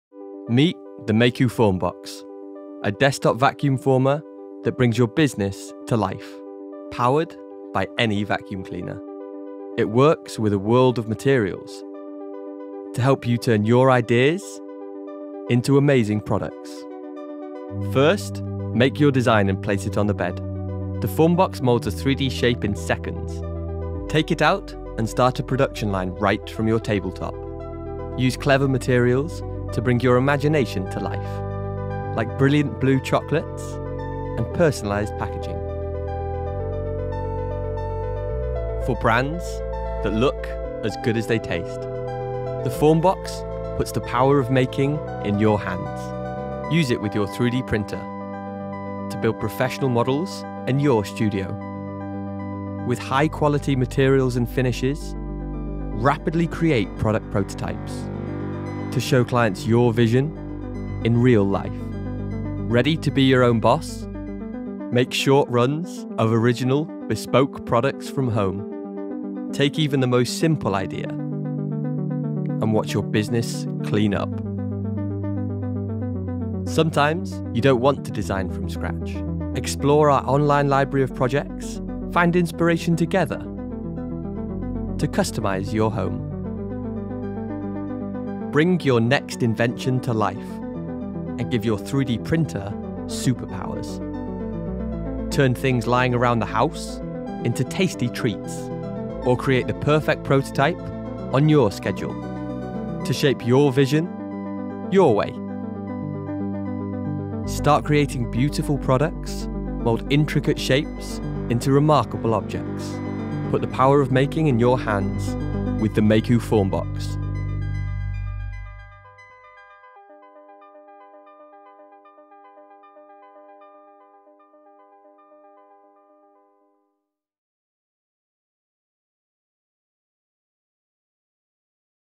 Mayku FormBox - Termoformatrice desktop 3DiTALY
3DiTALY introduce nel suo parco macchine la nuova Mayku Formbox, una piccola termoformatrice che racchiude una grande potenza. Si tratta di una termoformatrice formato desktop che imprime stampi su fogli riscaldati di polimeri grazie alla forza dell’aria aspirata (vacuum). Compatta e facile da usare Mayku FormBox rende la realizzazione di calchi e stampi un processo veloce ed accessibile a tutti.